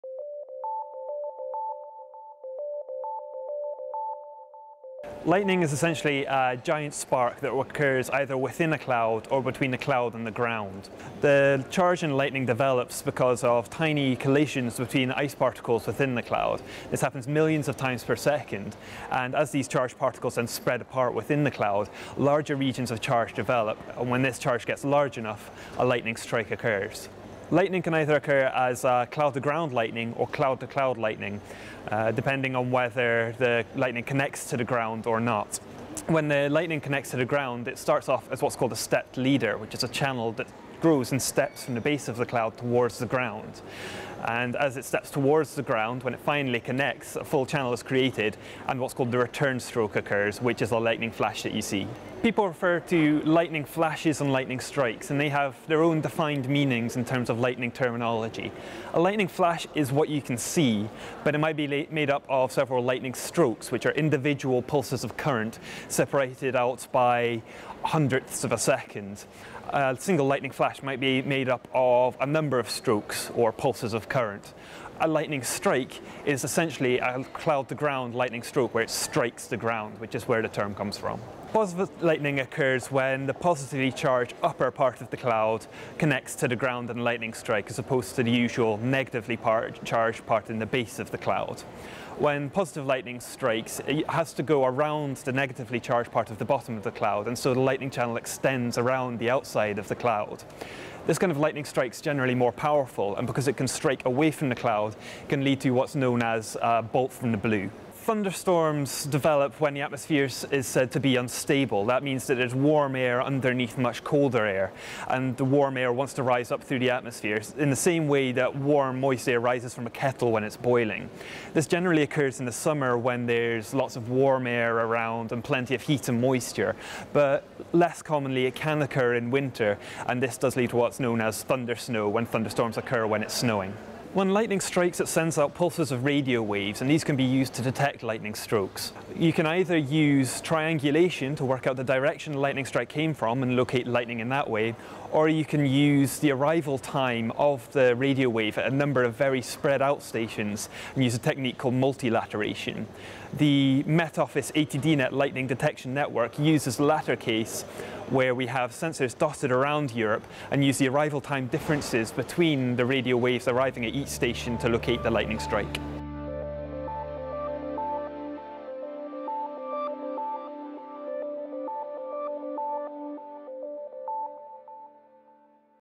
How does lightning form and how do we detect it?
In this video Foundation Scientist Graeme Anderson explains how lightning forms, different types of lightning such as thunder snow, and how we detect it.